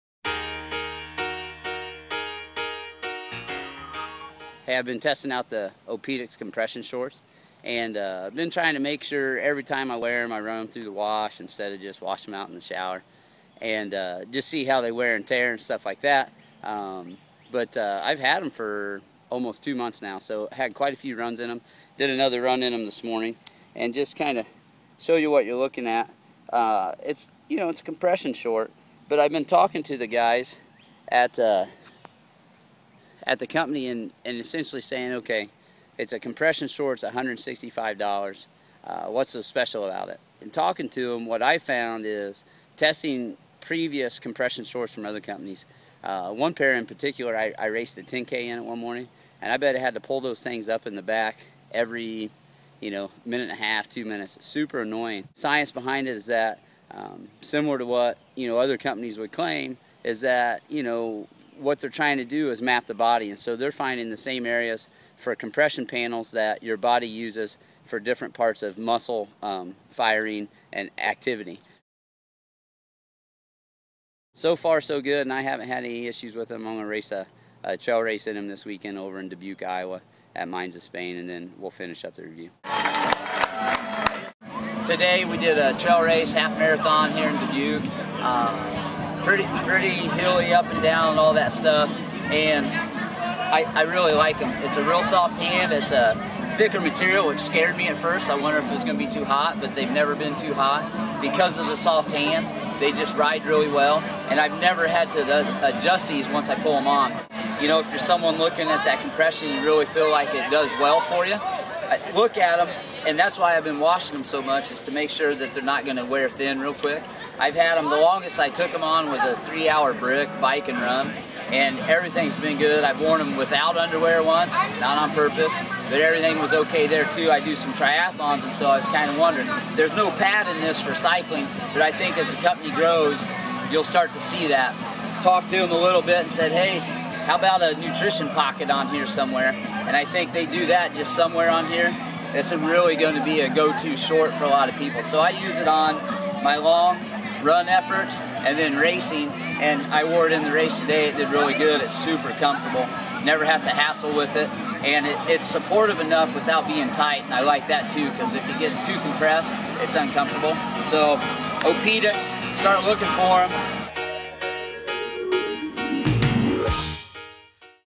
Opedix Compression Short Main Review
The Opedix Core-Tec Compression shorts have proven to be very comfortable, durable, and race worthy.  If your running leads you in search of running compression, you'd better have these as one of your possibilities.